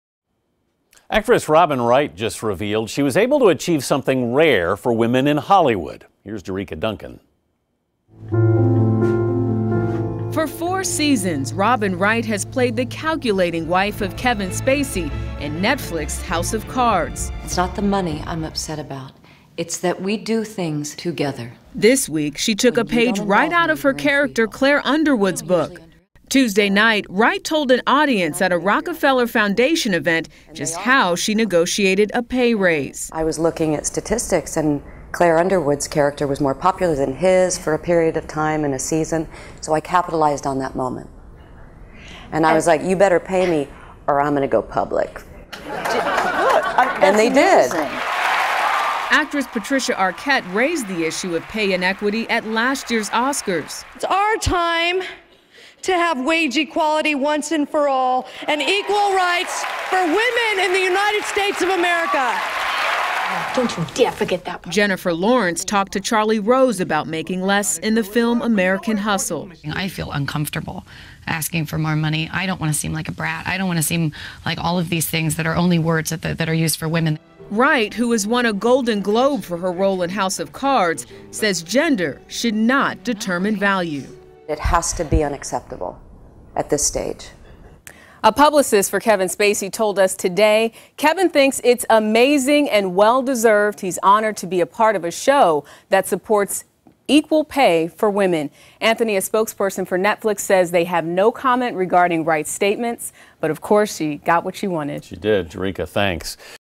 Robin Wright demanded pay equal to "House of Cards" co-star
Actress Robin Wright pulled a page out of her character Claire Underwood's playbook in salary negotiations for the Netflix drama "House of Cards." Wright demanded she be paid the same amount as co-star Kevin Spacey. Jericka Duncan has more.